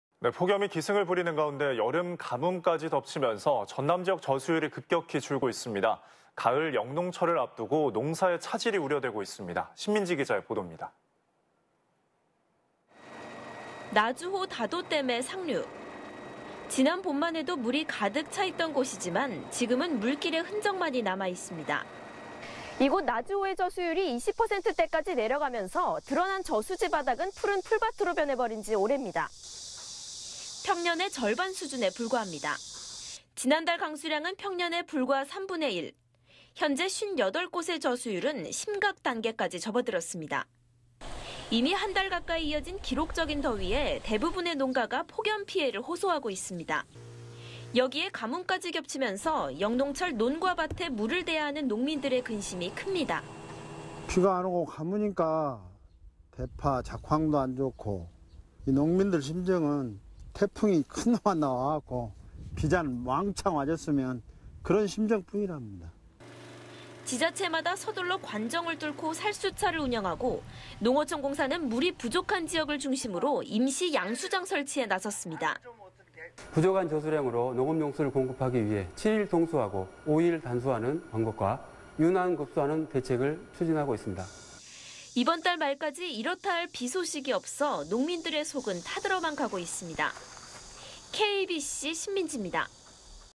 '폭염에 가뭄까지' 저수율 바닥..가을 농사 차질 '우려'
폭염이 기승을 부리는 가운데 여름 가뭄까지 덮치면서 전남 지역 저수율이 급격히 줄고 있습니다.

가을 영농철을 앞두고 농사 차질이 우려되고 있습니다.

신민지 기자의 보돕니다.